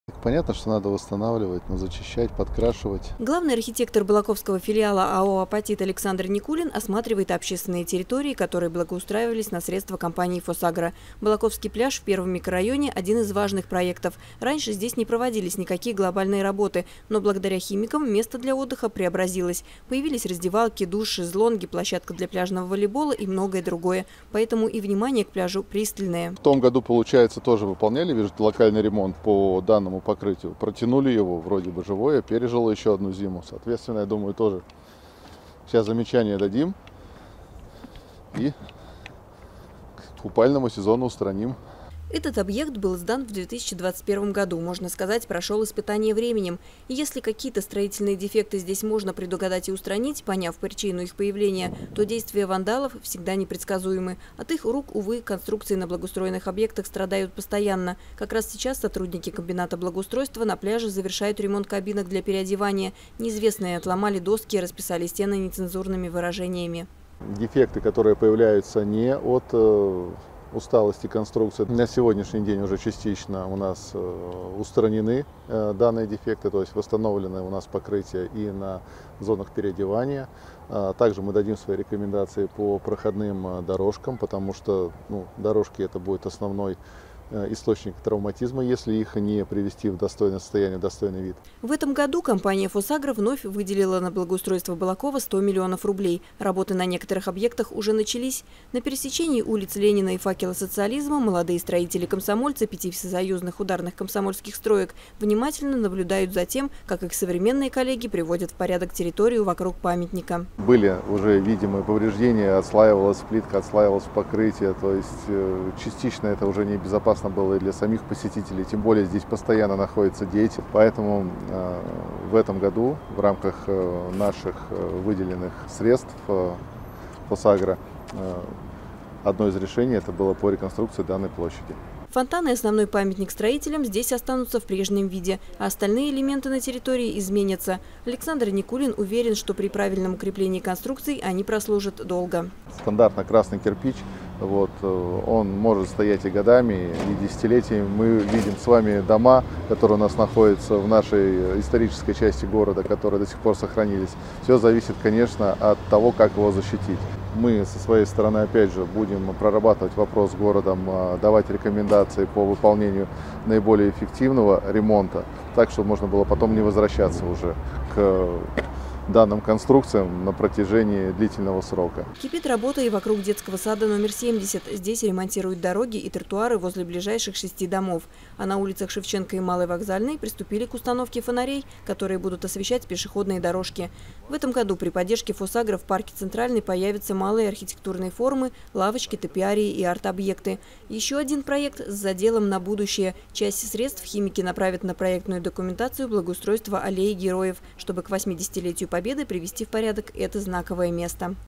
В Балакове начались работы по благоустройству городских территорий
В Балакове начались работы по благоустройству: в разных уголках города трудятся рабочие, им помогает спецтехника. Ежегодно приводить в порядок городские территории помогает компания «ФосАгро». За предыдущие три года она выделила на эти цели 370 миллионов рублей, в нынешнем году – ещё 100 миллионов. За проведением ремонта следят сотрудники отдела главного архитектора БФ АО «Апатит»: выявляют недочёты, помогая подрядчикам качественно выполнять работу. Подробности в сюжете